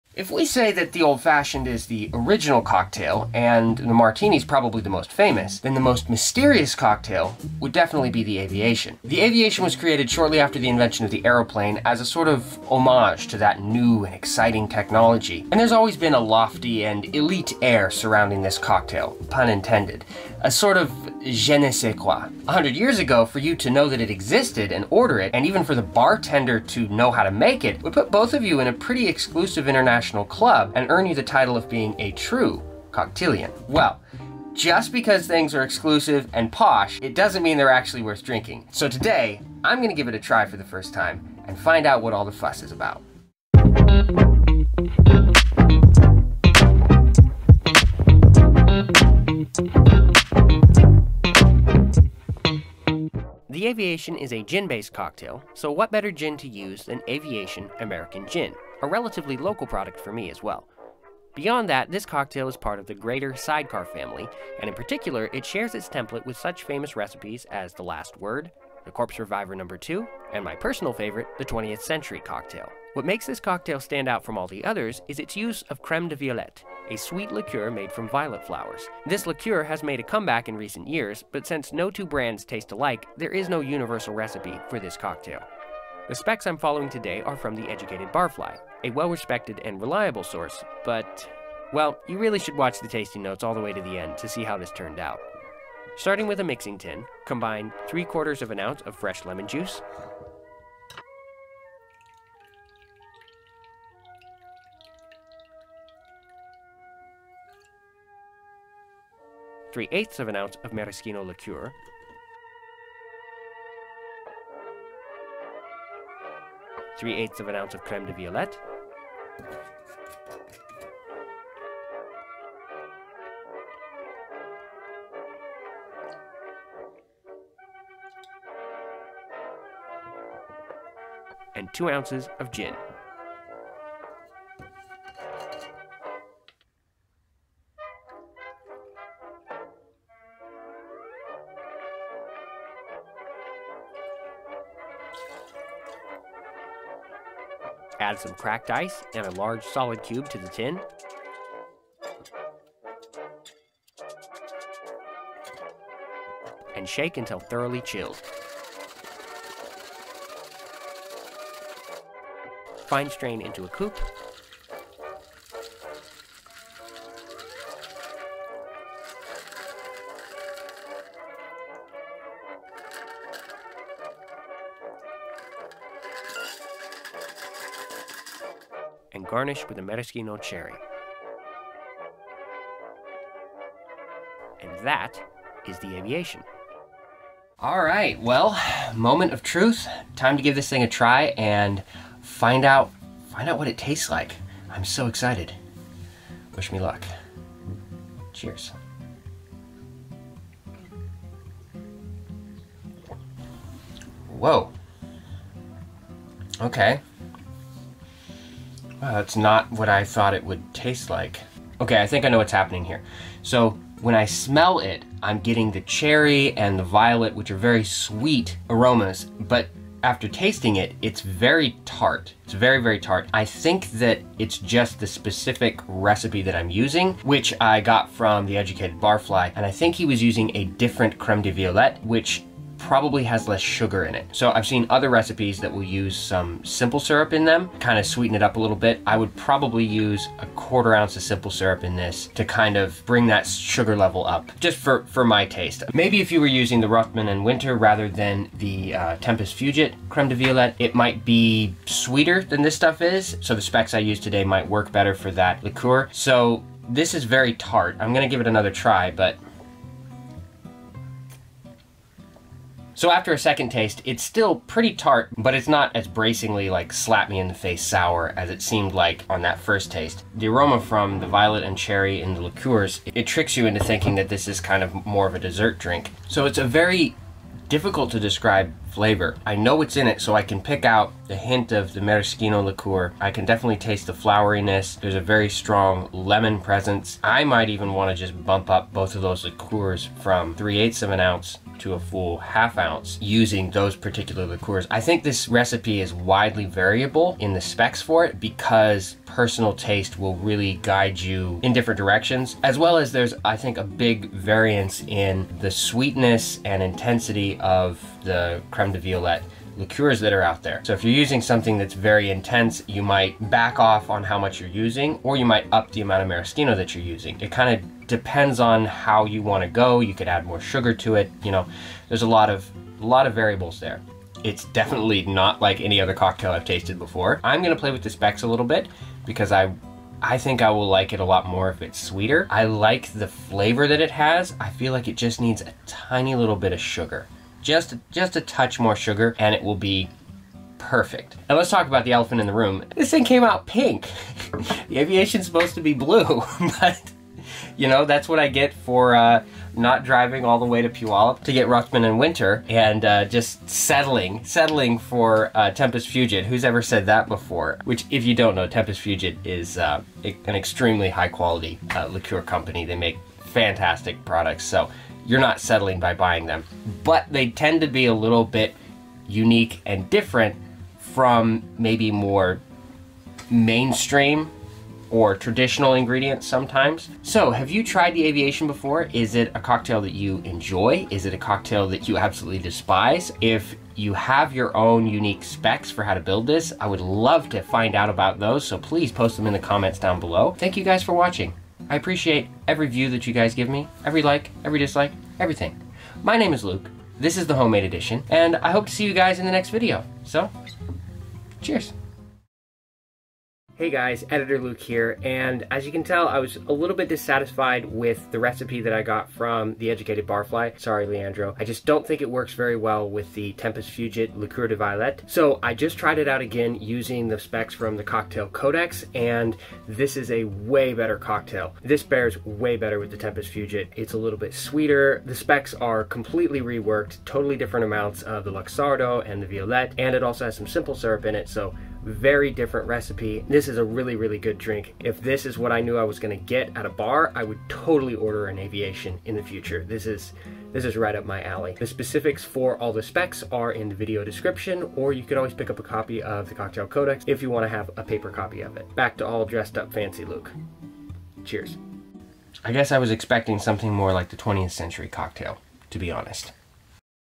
The Aviation Cocktail || The Educated Barfly vs Death and Company - which recipe is best?
The Cocktail CODEX recipe:
2 ounces Gin
¼ ounce Creme de Violette
1Tsp Maraschino Liqueur
¾ ounce Fresh Lemon Juice
½ ounce Simple Syrup
Shake with ice, serve up in a chilled coupe, garnish with a brandied cherry.

Amazon Music